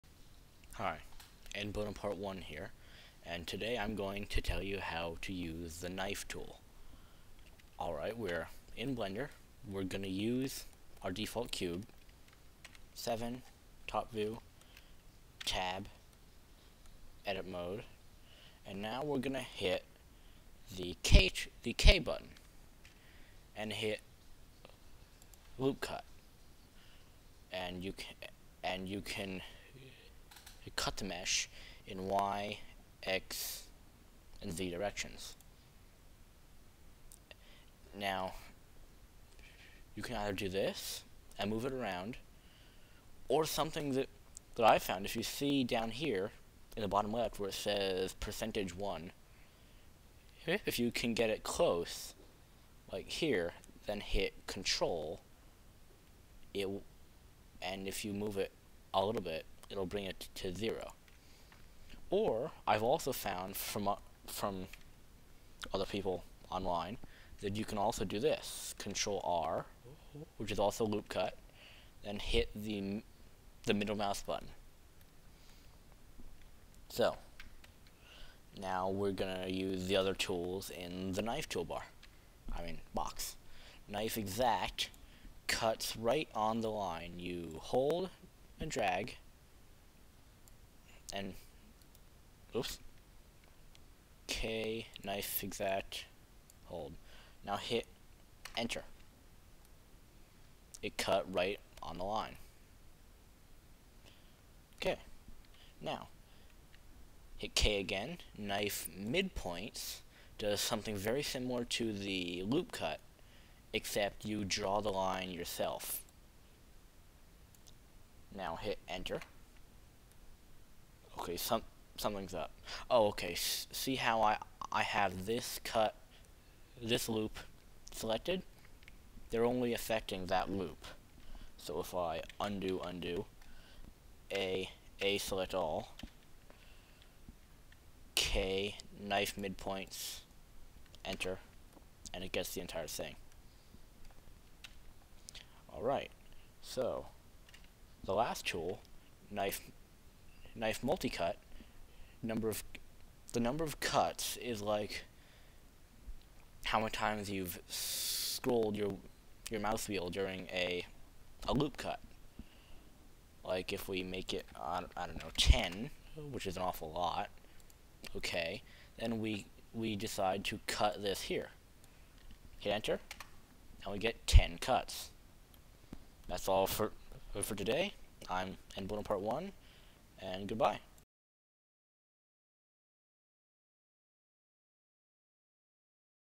Blender Knife Tool Tutorial
How to use Blender 3D's knife tool. Please excuse my stutter and lisp.